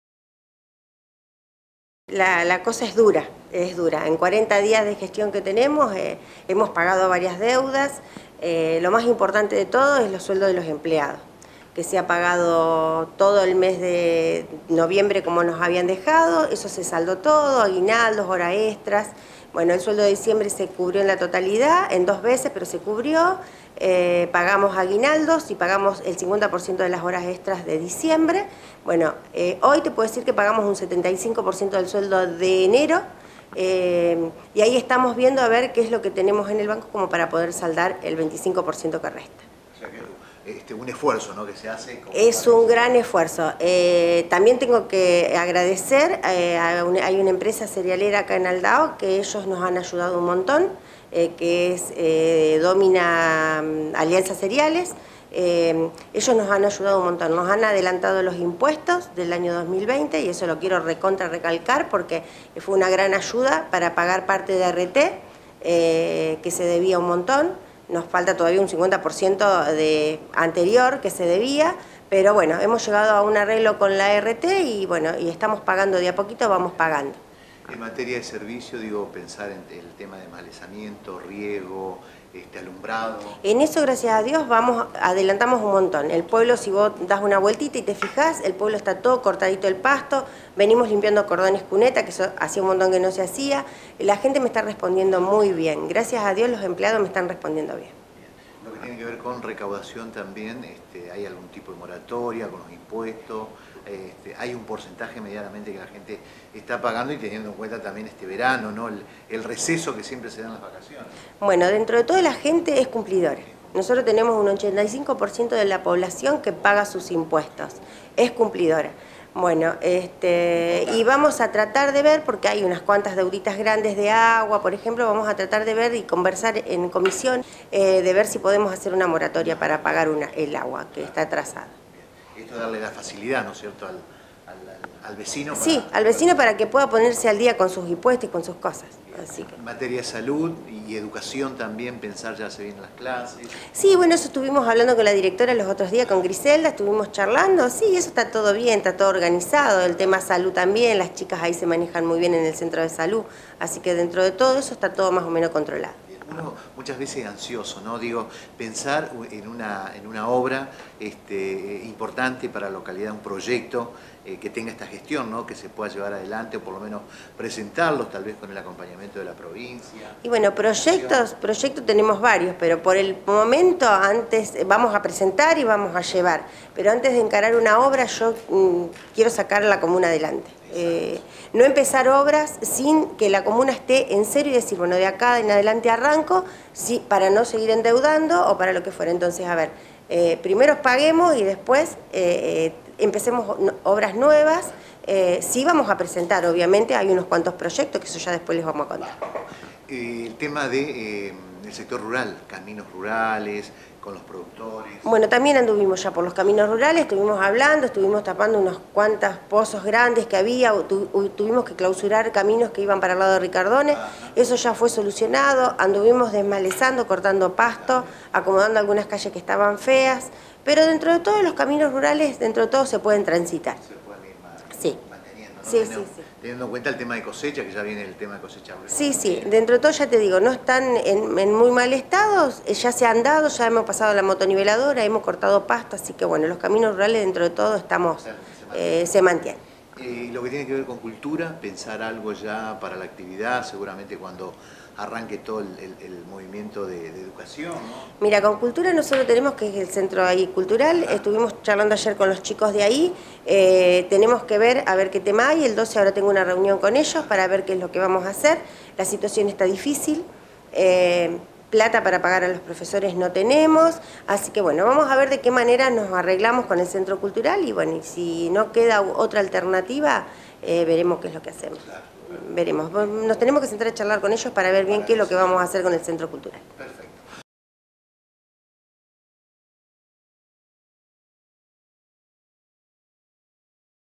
Situación financiera de la Comuna de Aldao
Claudia Maceratesi, presidente comunal de Aldao, se refirió al estado de las arcas financieras, que es delicado y afecta al inicio de los talleres del área cultural.